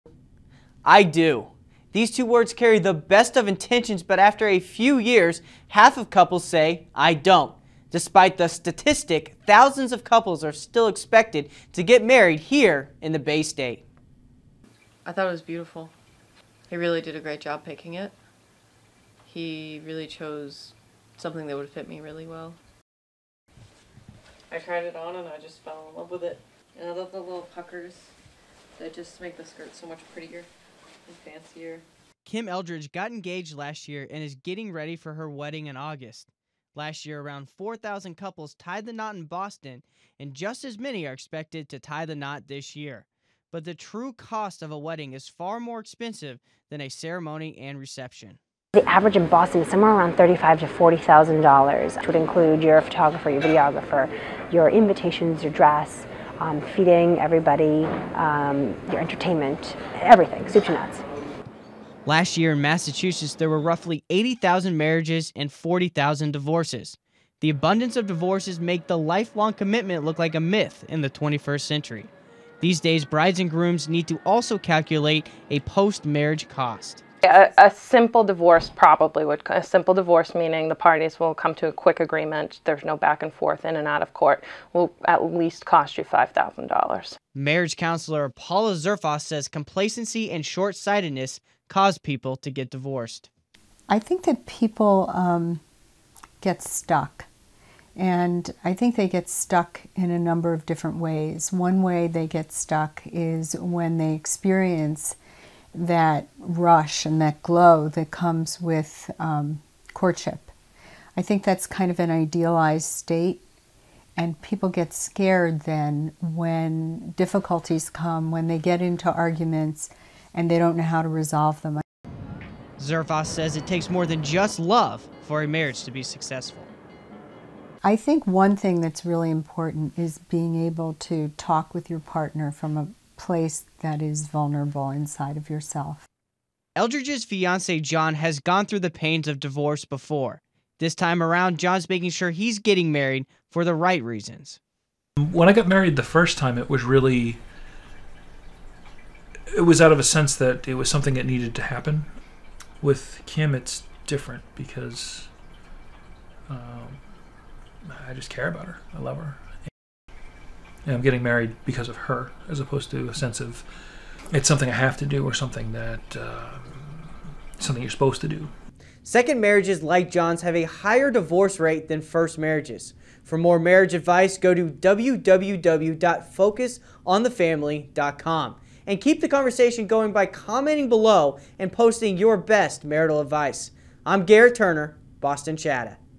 21st Century Marriage
Katie O'Donnell and Garrett Turner discover the new age definition of marriage and find that people are still have hope to keep their life time commitment.